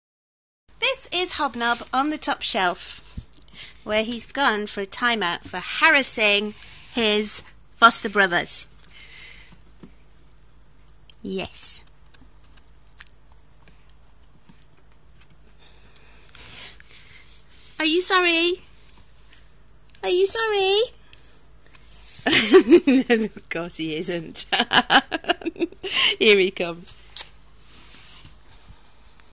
Ratty time out
Every time Hobnob harasses my rescue ratties (they were raised socially isolated and are scared of other rats still) he gets plopped on the top shelf for time out. Unfortunately he's too cute for me to punish *lol*